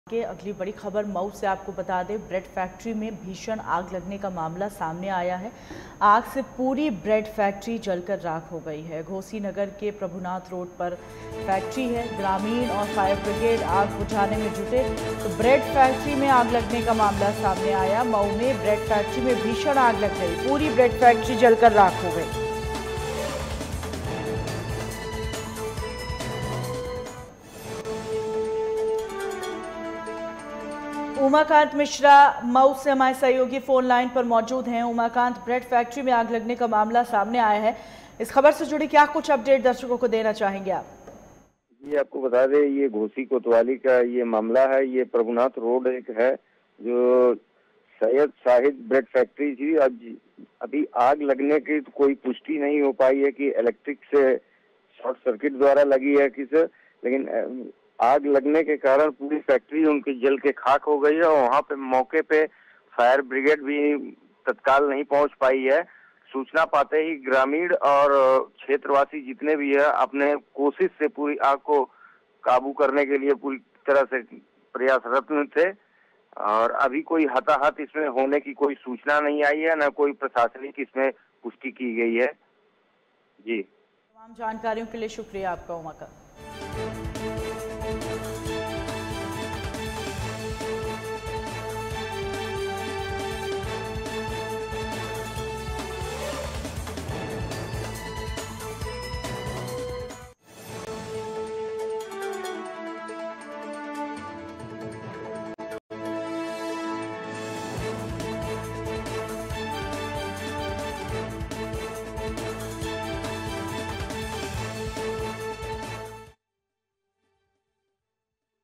Breaking News : Mau में ब्रेड फैक्ट्री में लगी भीषण आग
हमसे जुड़े रहने के लिए हमें फॉलो करें